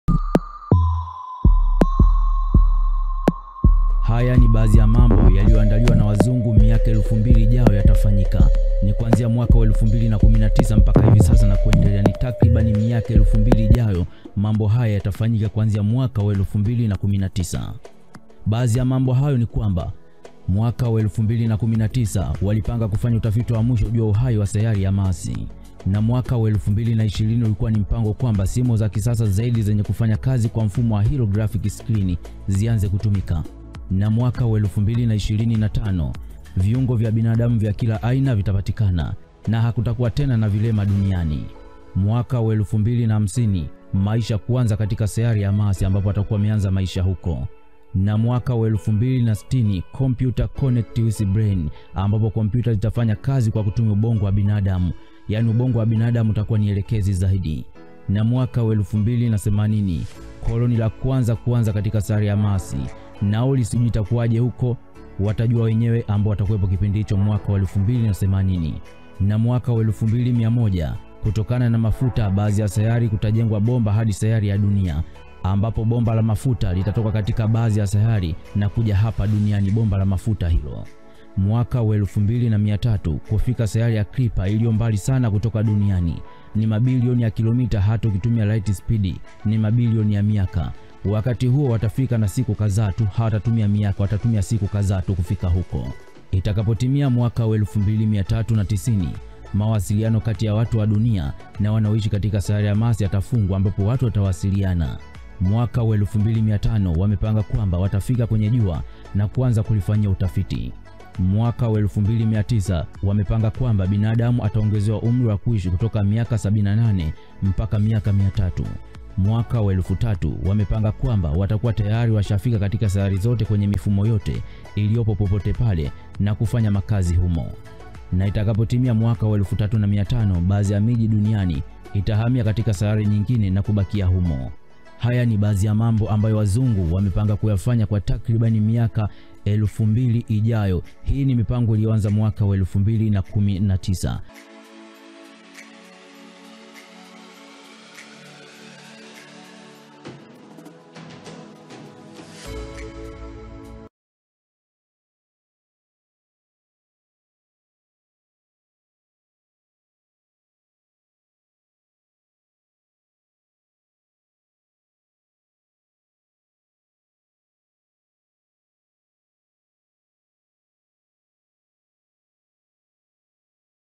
Staajabika na Mipango ya Wazungu Kupitia Wanasayansi Wao kwa miaka 2000 ijayo
Wanamitazambo ya Mbali na nimatumaini yetu hawata iangamiza Dunia kutokana na JARIBU JARIBU Yao.
- Wanataka Binadamu waanze kulipa NAULI kwenda sayari ya mars
- wanataka wawe wanasafirisha mafuta na mali ghafi nyingine kutoka duniani kwenda sayari nyingine kwa kutumia Mabomba
- Wanataka kuunda viungo bandia vya mwili wa binadamu na vikiwekwa mwilini viwe kama viungo Kamili